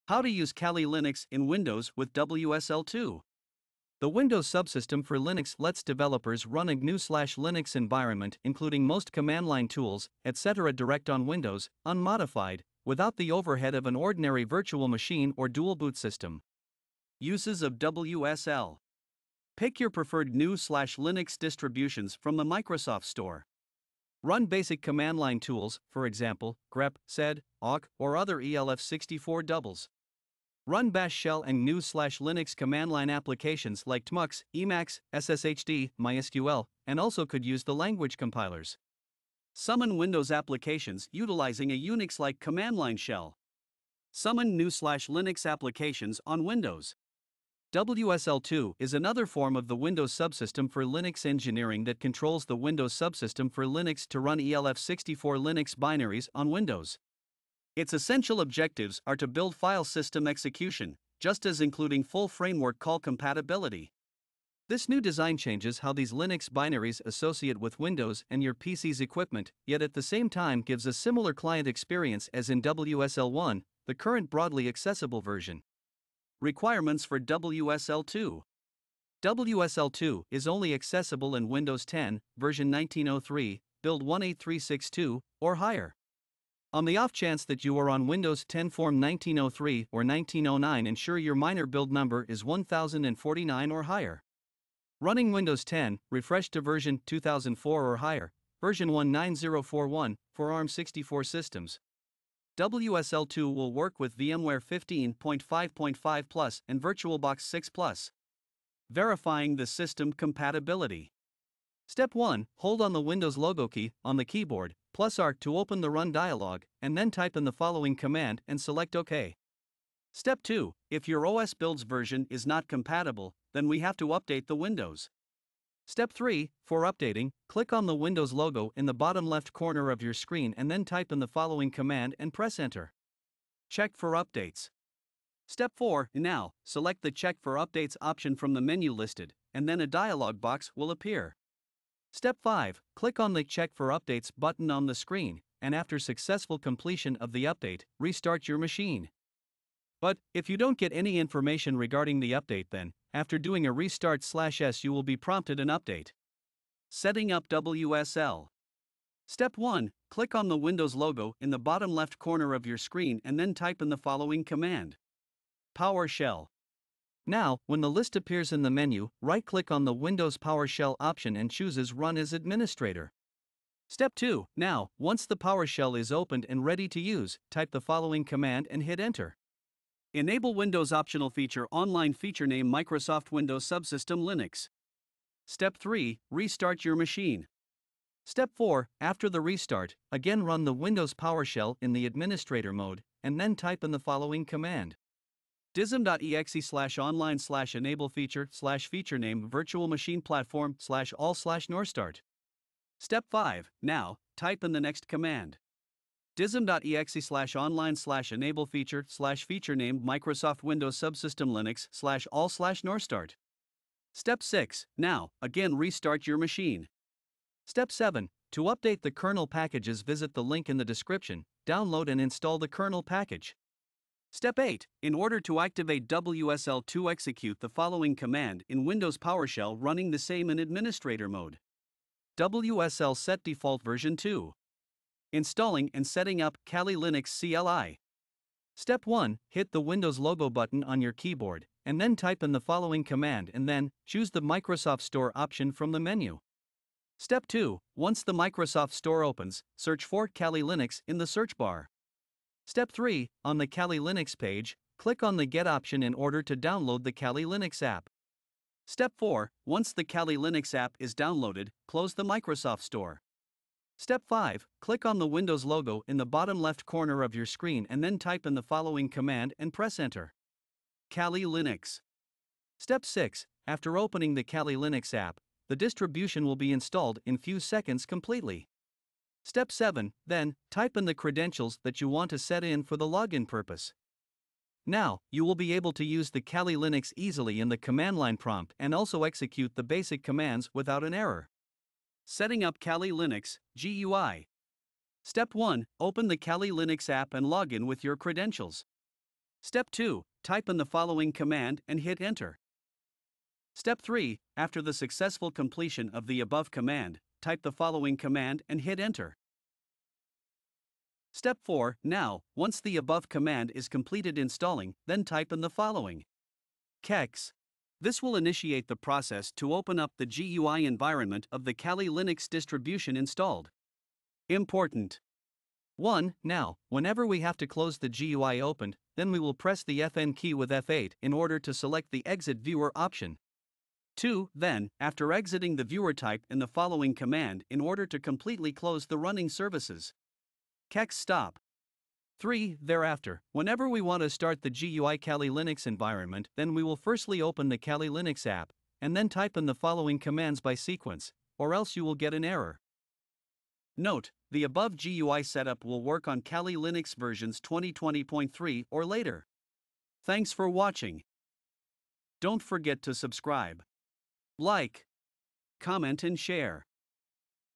How to use Kali Linux in Windows with WSL 2?
In order to update the kernel packages download the kernel package and install the same.